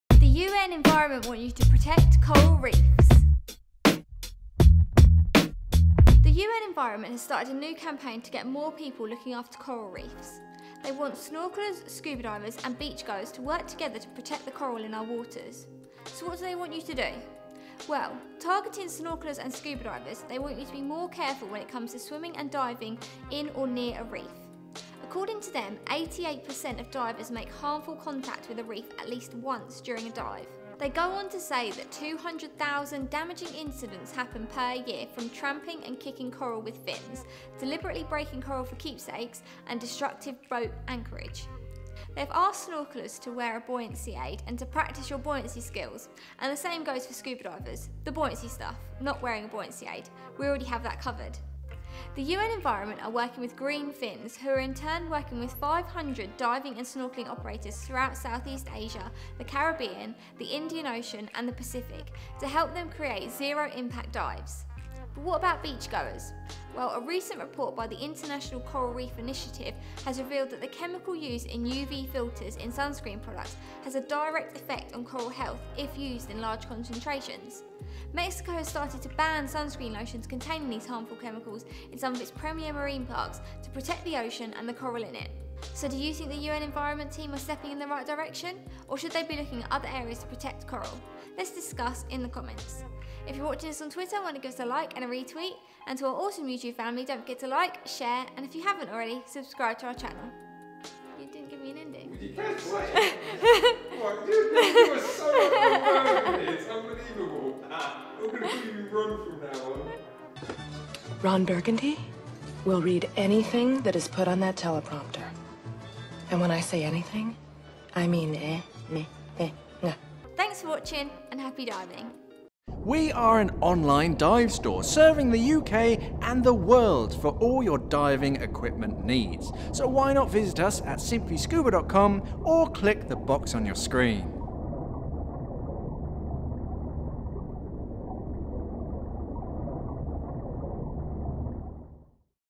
Daily Scuba News - The UN Environment Want You To Protect Coral Reefs
The UN Environment Want You To Protect Coral Reefs

  The UN Environment has started a new campaign to get more people looking after coral reefs. They want snorkelers, scuba divers and beachgoers to work together on protecting the corals in our waters, So what do they want you to do?

Well, targeting snorkelers and scuba divers they want you guys and girls to be more careful when it comes to swimming a diving in or near a reef. According to them, 88 percent of divers makes harmful contact with a reef at least once during a dive.

::Contributors::
- UN Environment
- Coral Reef Image Bank
- Robyn Caldwell
- Mark Newman
- Shaun Johnson